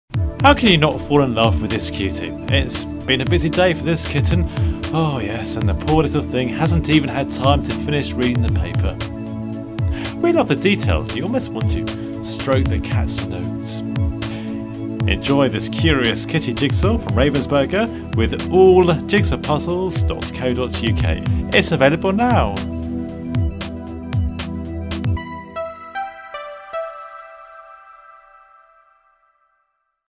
Curious Kitty | Ravensburger
Catching up on the news, this cute little kitten looks ready to take a break and play!